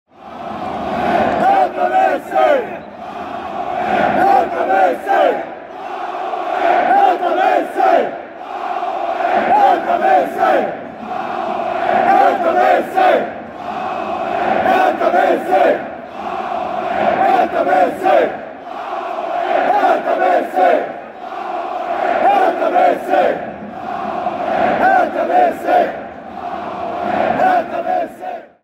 HA HO HE HERTHA BSC - Wechselgesang beim Bundesliga-Abschied in Wolfsburg!
34. Spieltag, Saison 2022/23: VfL Wolfsburg - Hertha BSC 1:2